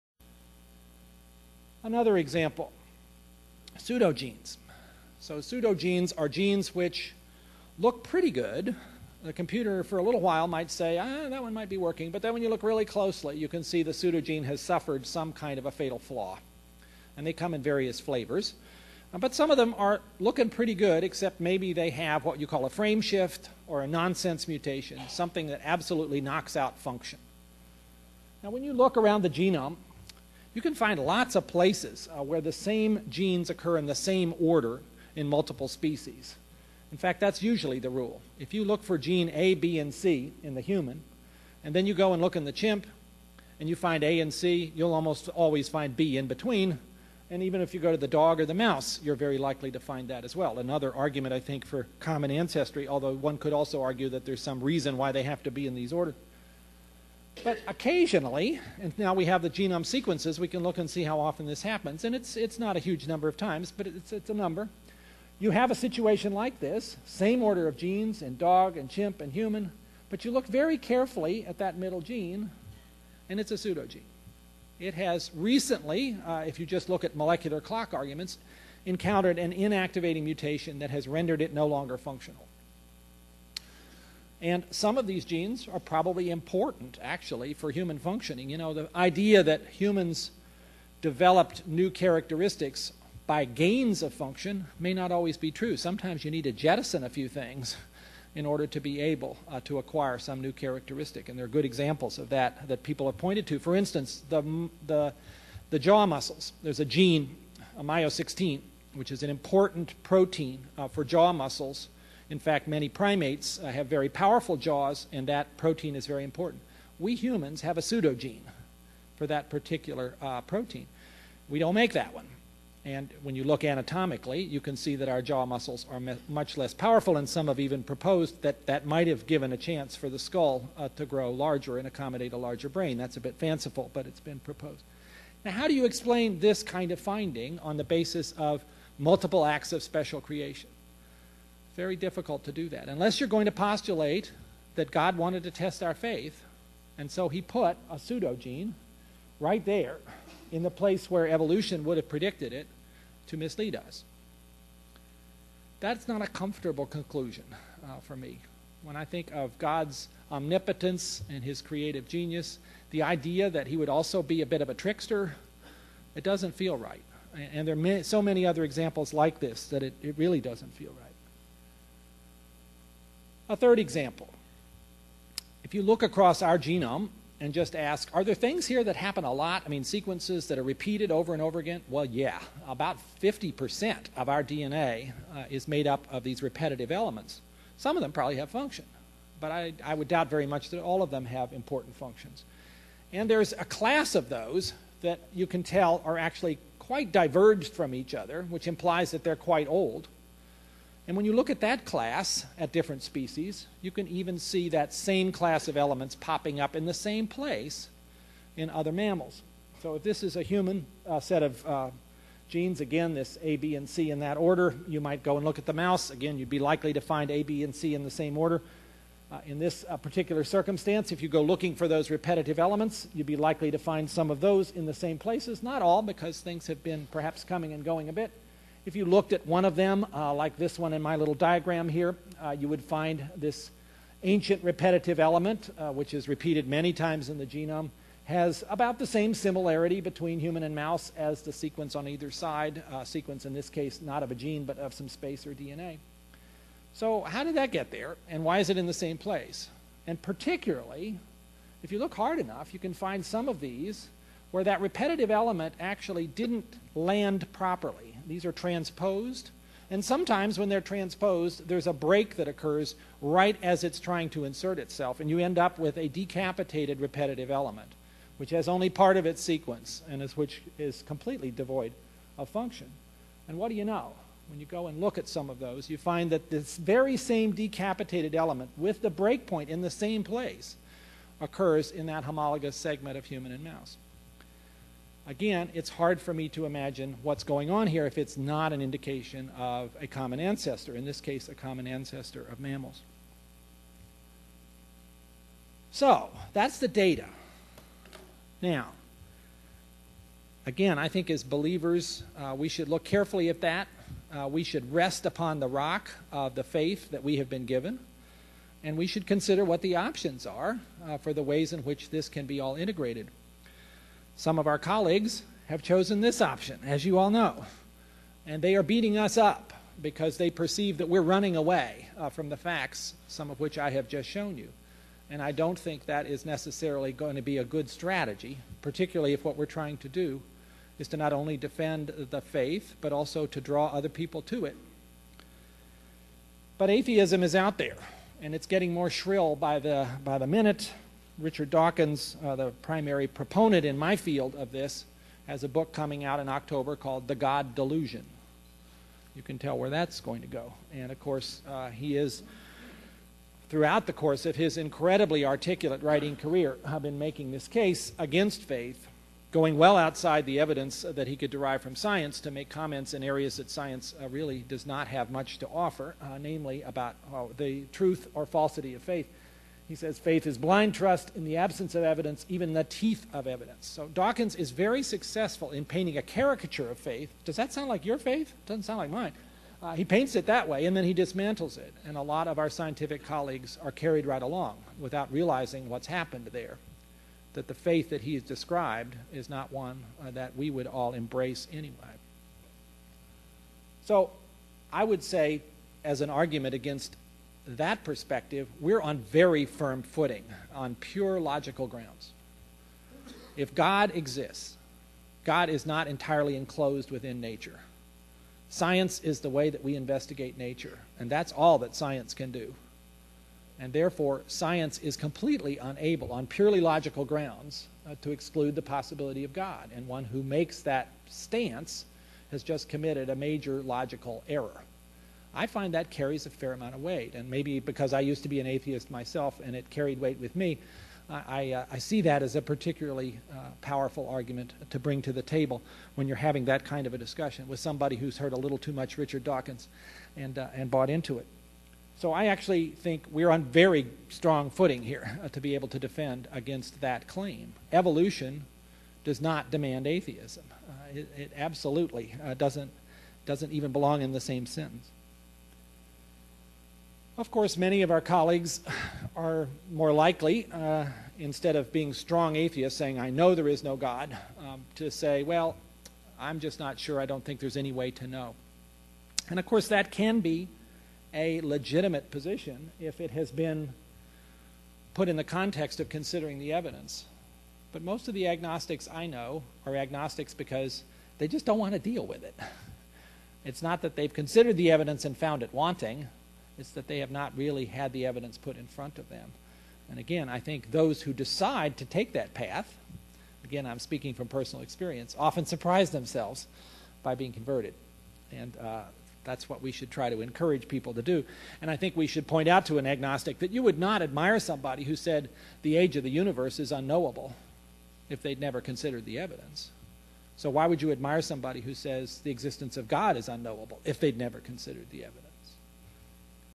Francis Collins The Language Of God 5of9
Francis Collins of the Human Genome Project discusses evolution, creationism, Intelligent Design and the harmony that can exist between faith and science.